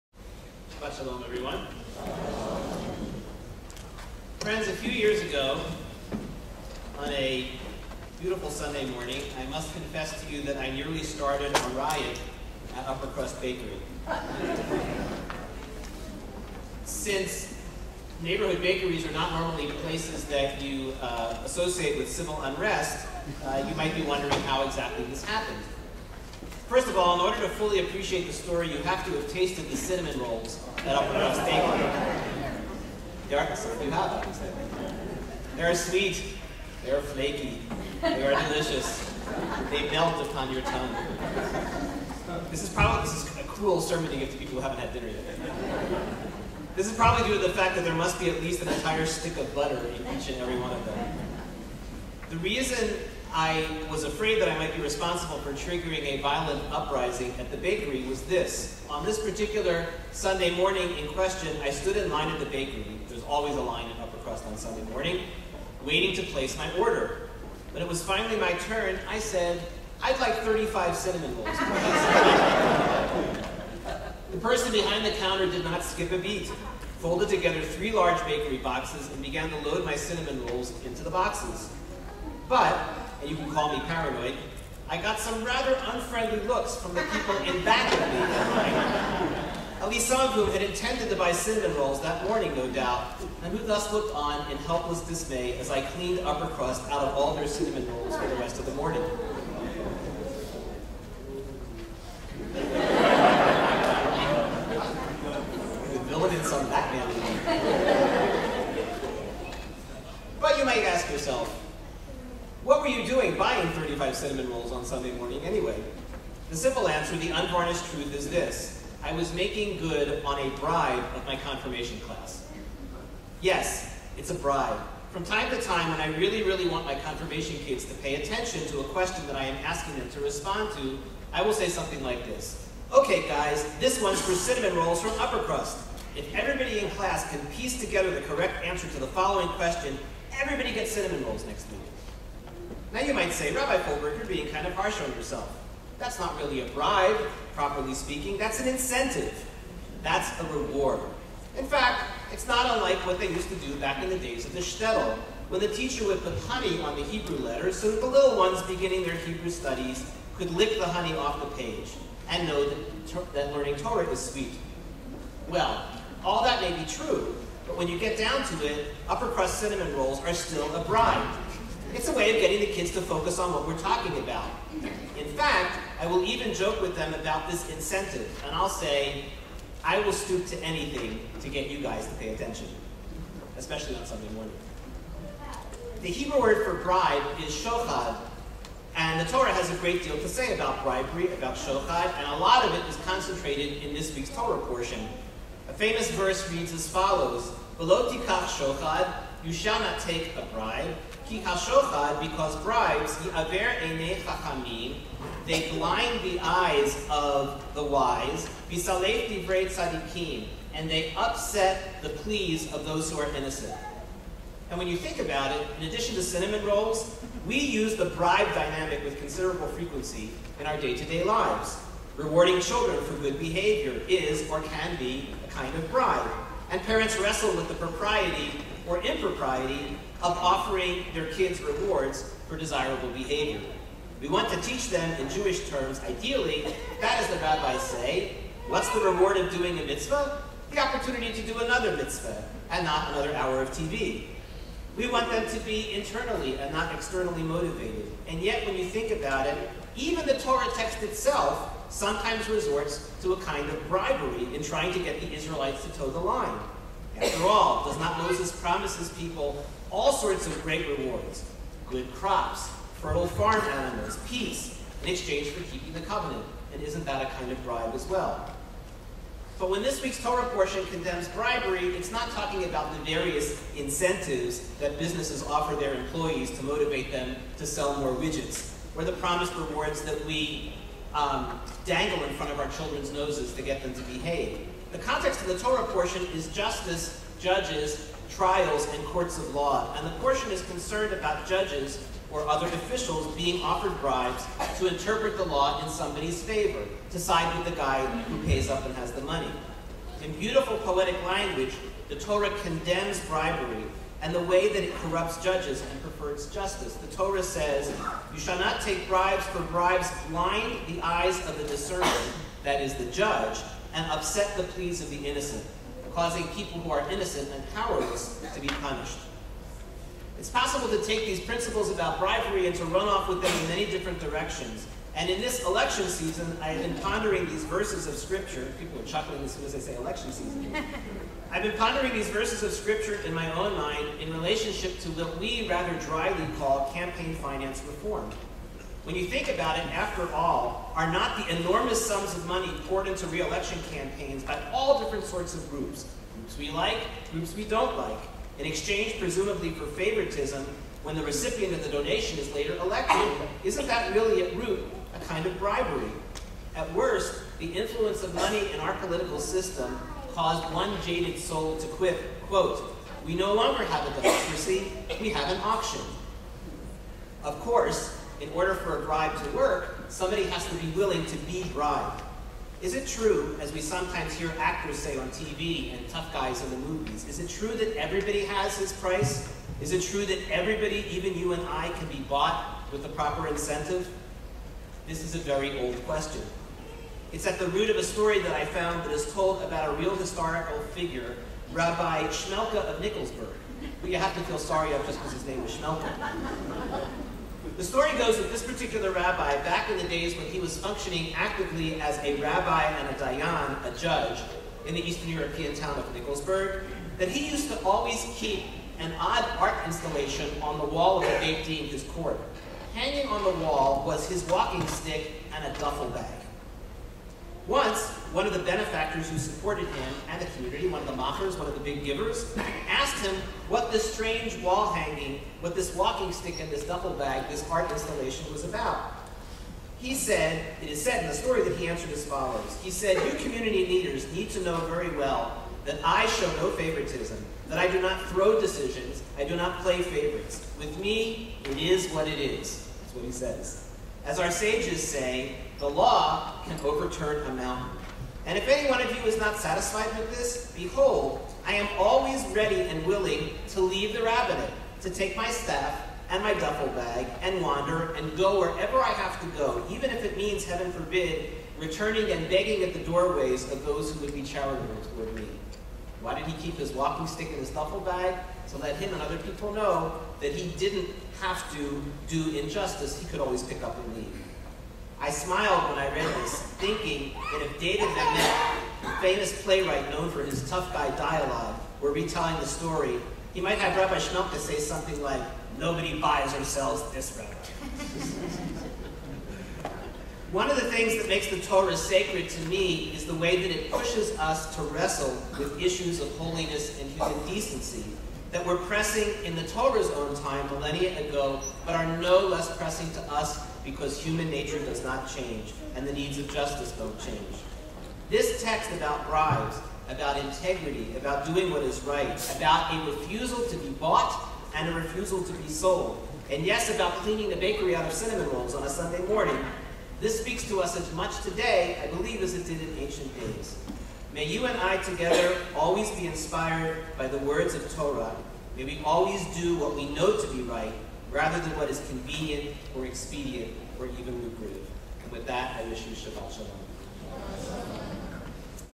Shabbat Sermon - 09/05/2008
Rabbi Steve Folberg sermon, Parasha Shoftim
Congregation Beth Israel, Austin, TX